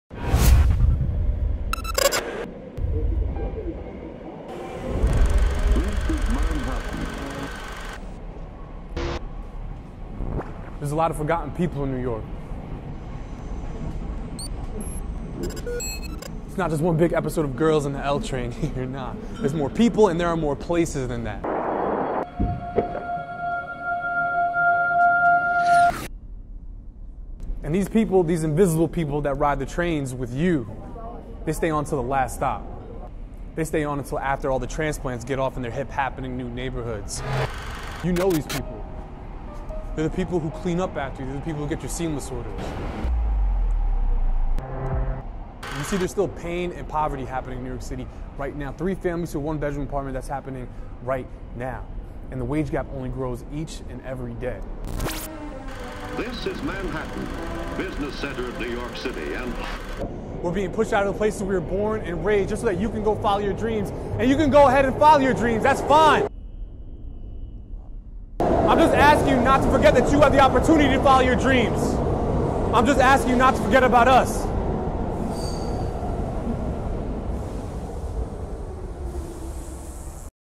Don't Forget About Us
Thanks to helping me film: @michaelpatras
Gentrification is happening all across New York City at the moment. 80/20 apartments are proving to be not enough and the conversation about rent has stalled. We need to start these conversations again.